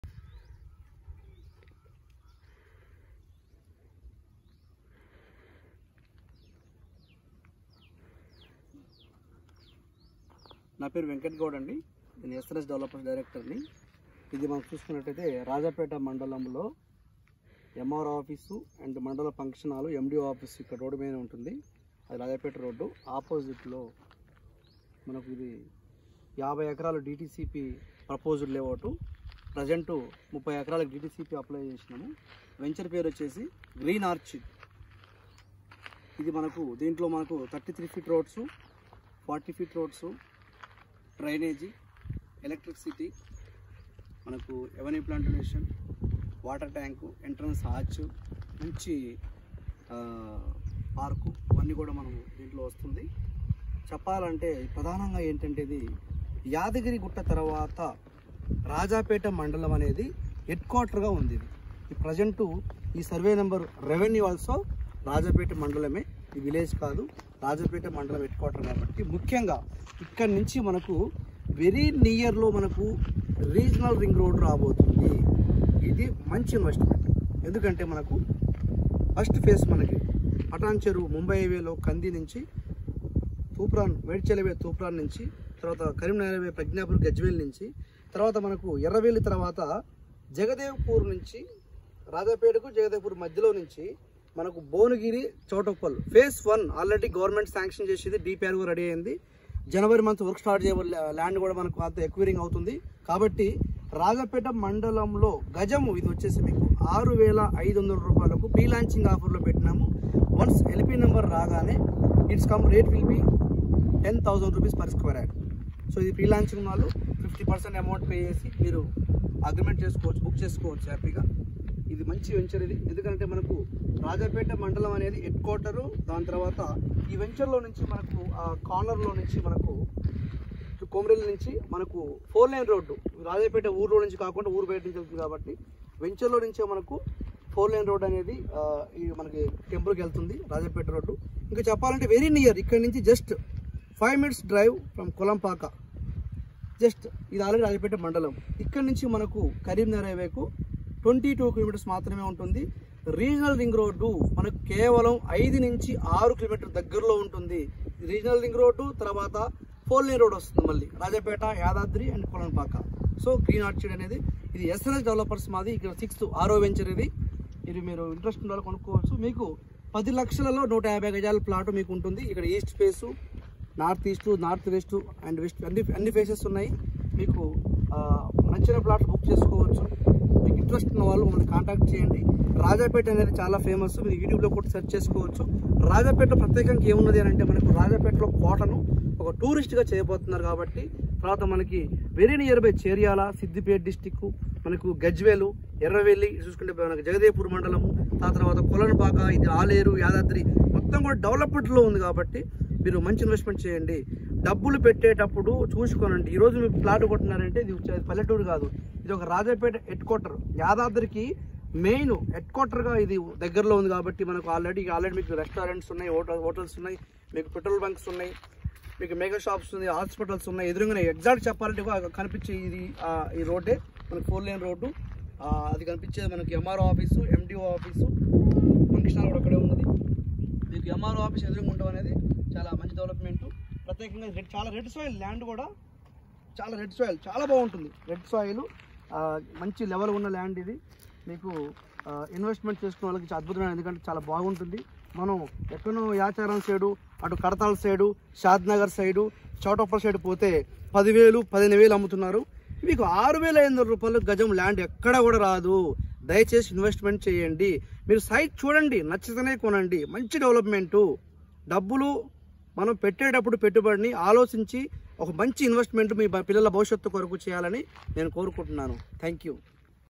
Green Orchid SNS developers Dtcp purposed layout 50 acres at Rajapet mandal Head Quarter
Green orchid SNS developers per sqrd 6999/-  Rajapet mandal head quarter opp Mro & mdo office  very near to Regional Ring Road interested customers call or Whtsapp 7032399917